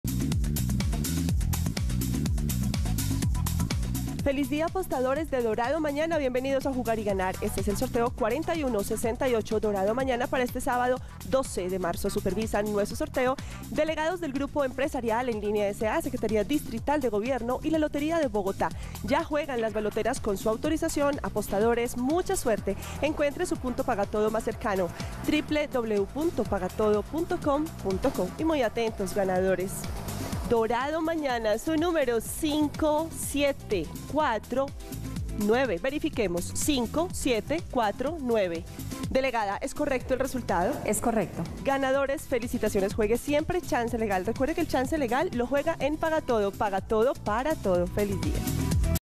EL DORADO MAÑANA Resultado del SÁBADO 12 de marzo de 2022 ✅🥇🔥💰
Hola buenos días, te presentamos los resultados de las loterías y chances en Colombia, vamos con el Resultados del Chance Dorado mañana del SÁBADO 12 de marzo de 2022.